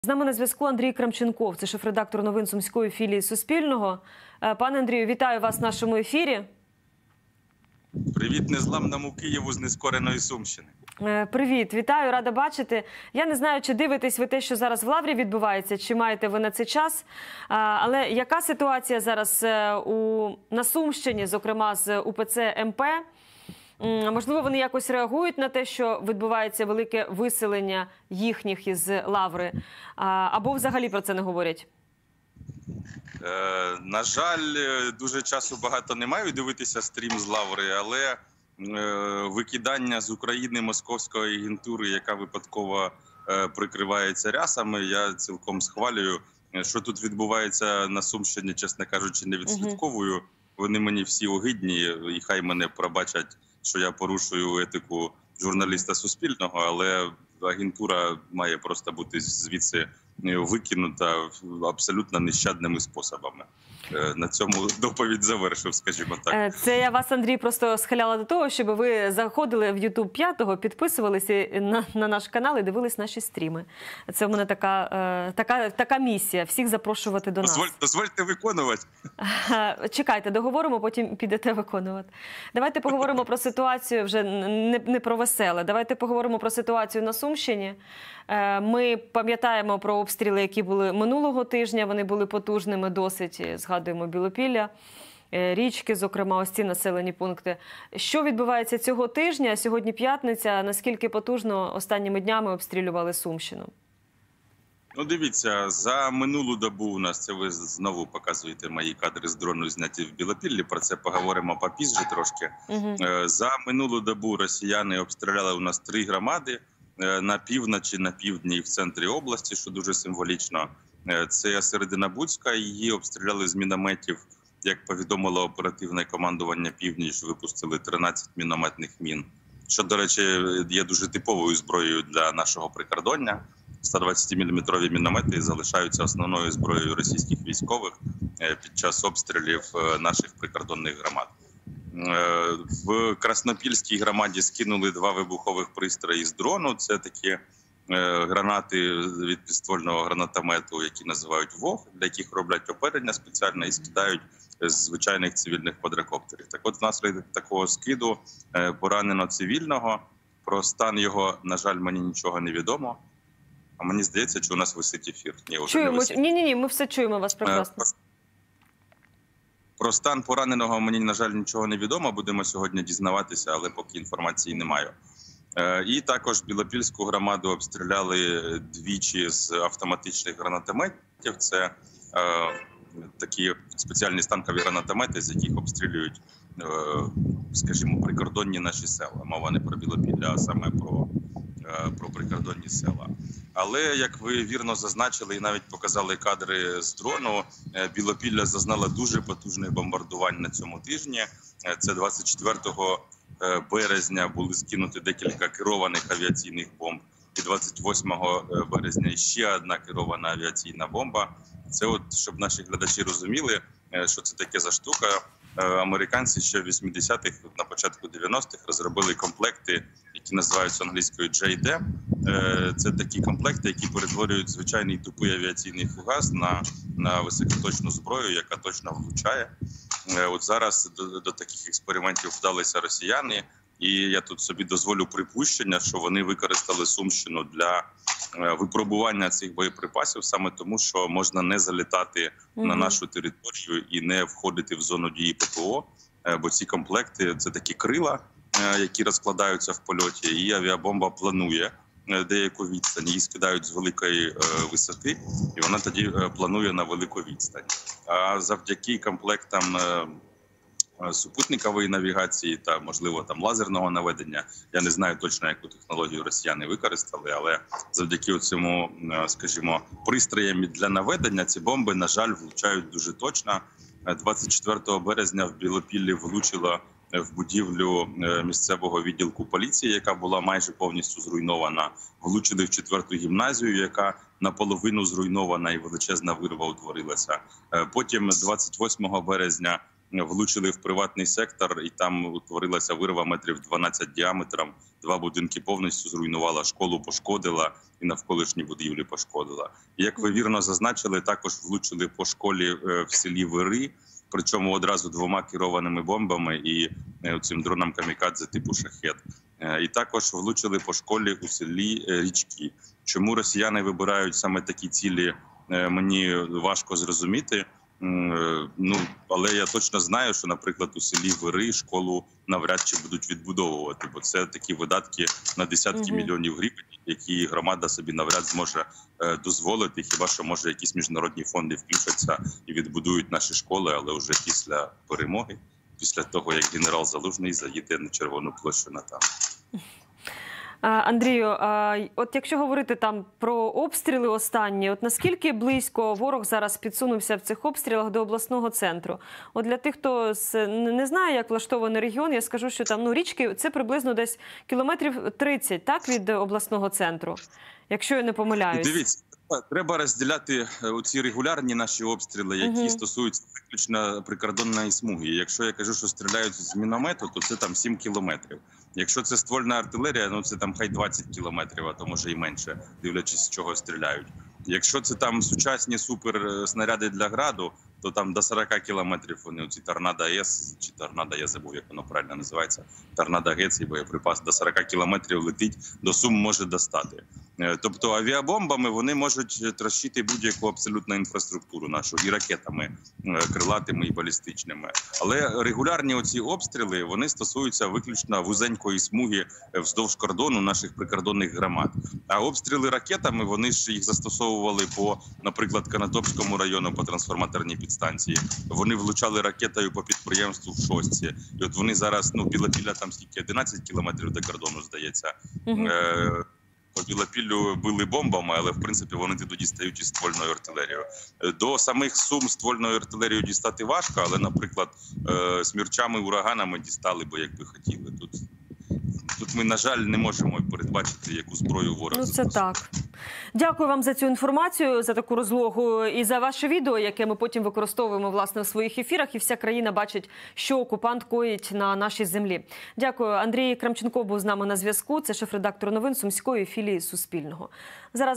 мОСКОВИТИ з ДРОНА скинули вибухові пристрої: ситуація на Сумщині – Андрій Крамченков докладно
Андрій Крамченков, шеф-редактор Сумської філії "Суспільного"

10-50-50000 - будь-яка сума має значення.
Разом переможемо!
Слава Україні!